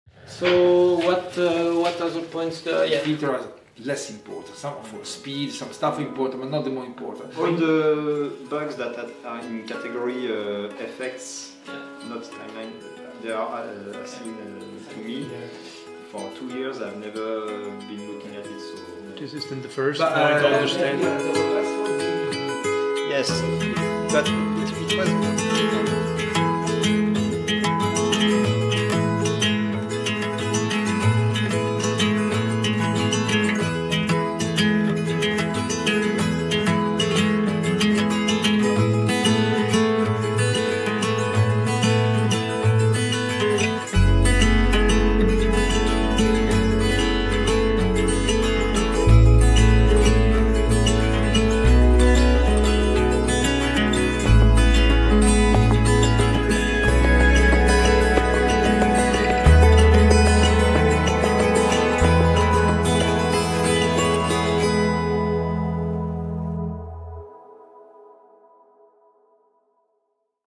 Kdenlive Sprint 2019 - Day 1
The Kdenlive team is sprinting, pushing forward the development of KDE's video editing software. They made this clip of what happened on day 1.